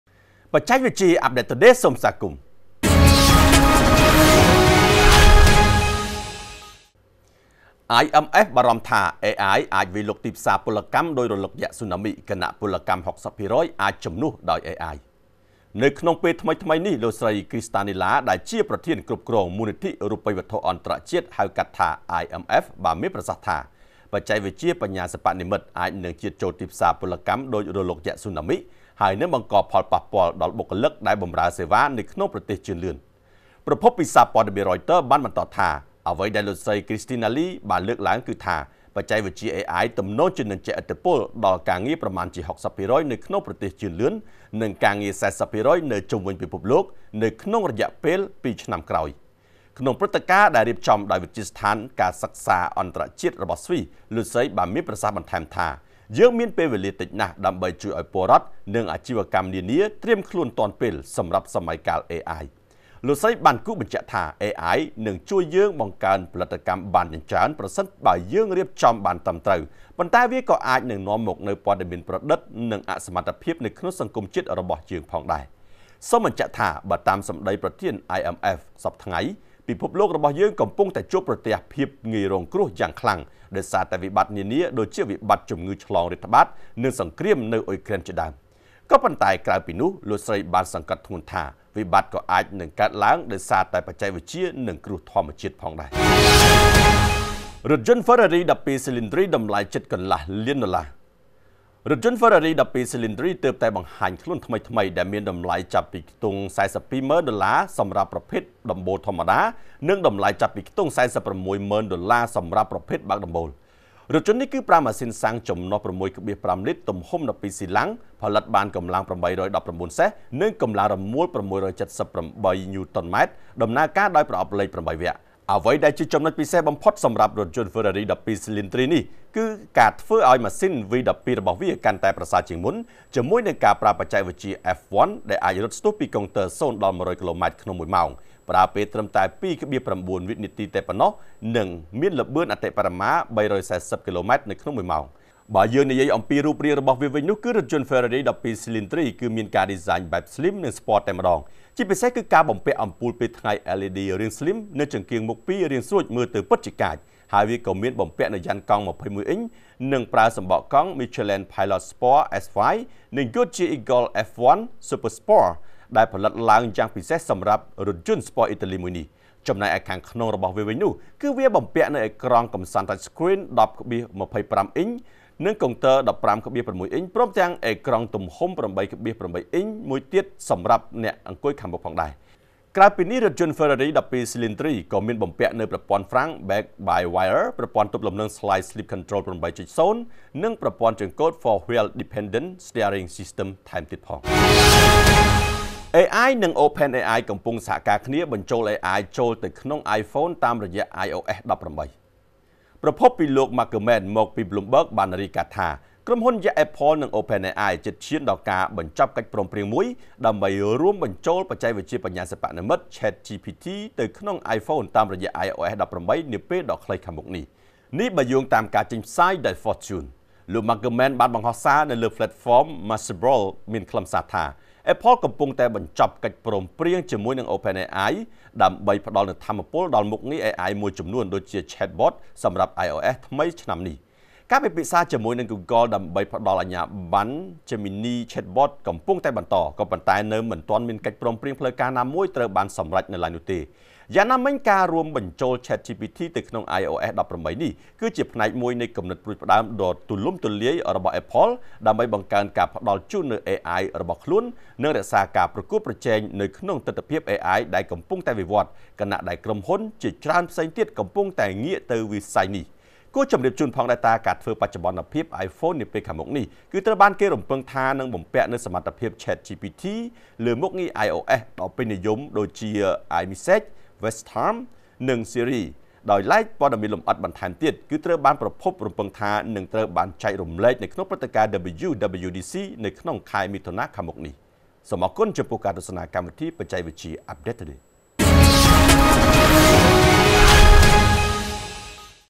IMF បារម្ភថា AI វាយលុកទីផ្សារពលកម្មដូចរលកយក្ស ស៊ូណាមី ខណៈ....| UPDATES TODAY (14-05-2024) 2PM
▪ IMF បារម្ភថា AI វាយលុកទីផ្សារពលកម្មដូចរលកយក្ស ស៊ូណាមី ខណៈពលកម្ម ៦០ភាគរយអាចជំនួសដោយ AI
▪ ឡាន Ferrari 12 Cilindri  តម្លៃជិតកន្លះលានដុល្លារ
▪ Apple និង OpenAI កំពុងសហការគ្នាបញ្ចូល AI ចូលទៅក្នុង iPhone តាមរយៈ iOS 18